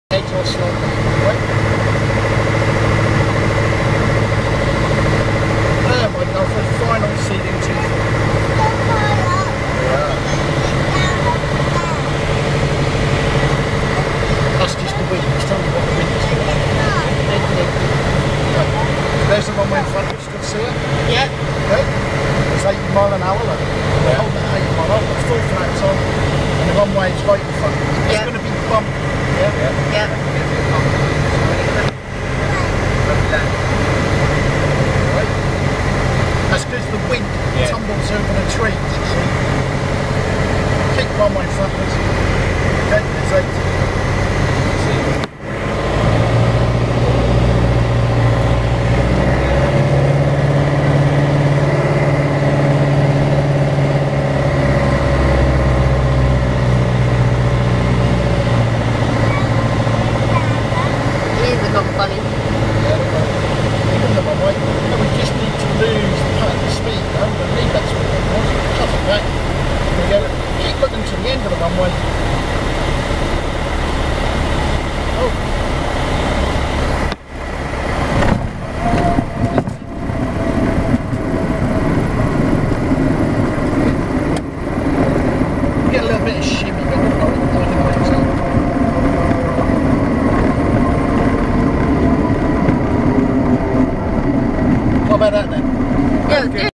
The landing at Seething after Hayley's flying lesson
Seething, around Lowestoft and Back to Seething